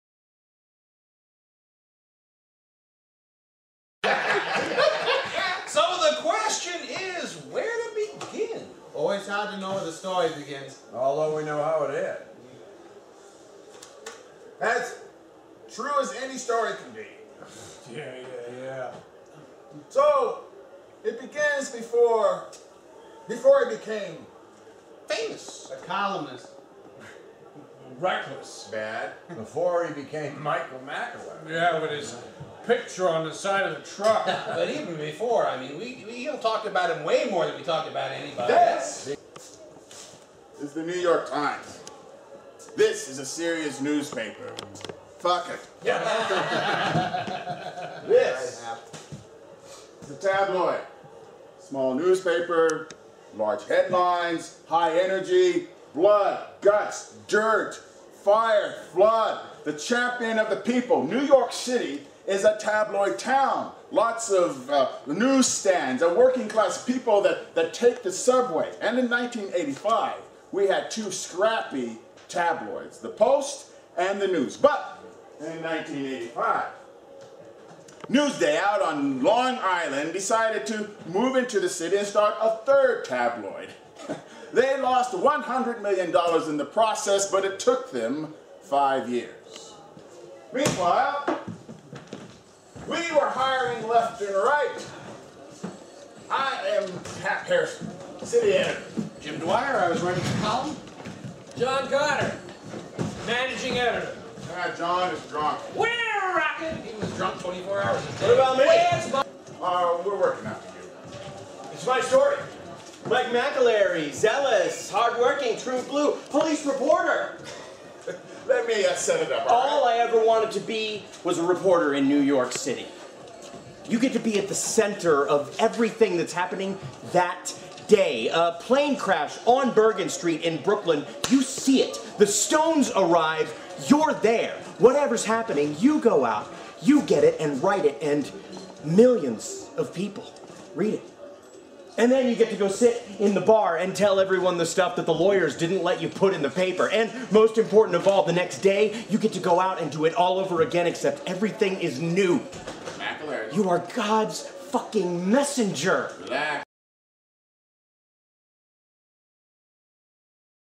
The Edge Theater presents "Lucky Guy" June 12 - July 5, 2015
Lucky Guy depicts the story of journalist Mike McAlary beginning in 1985 and ending with his death at the age of 41 in 1998. The plot covers the high points and tribulations of McAlary’s career as he traverses the clubby atmosphere of the New York City tabloid journalism industry prior to the 24-hour news reporting cycle.

The Edge Theater presents the Regional Premiere of "Lucky Guy" by Nora Ephron June 12 through July 5 at 1560 Teller Street in Lakewood. Performances are Thursday, Fridays and Saturdays at 8 p.m.; Sundays at 6 p.m. Tickets are $26 and available or by calling .